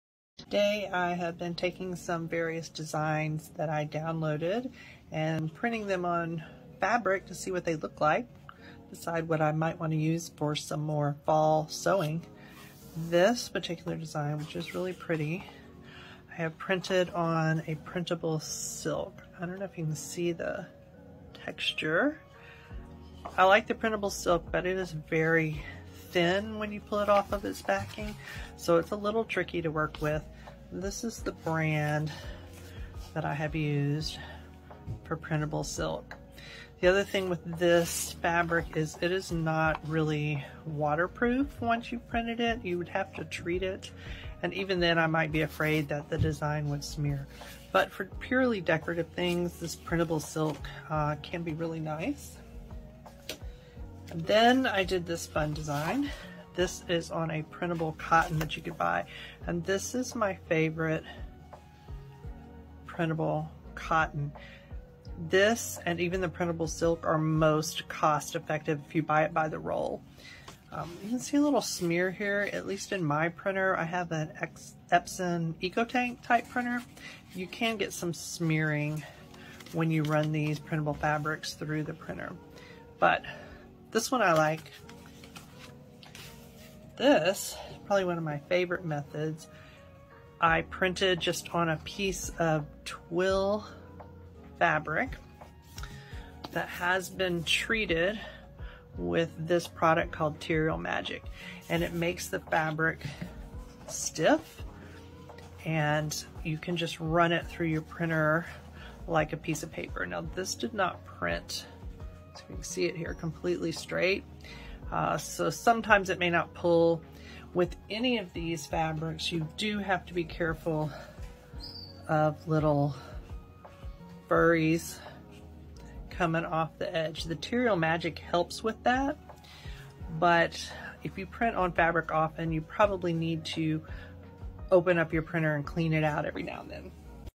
Sometimes I print on fabric for immediate use, but often I audition a design before sending it to t
Sometimes I print on fabric for immediate use, but often I audition a design before sending it to to print. I really like the first design! Now what should I make with it? All designs from CreativeFabrica.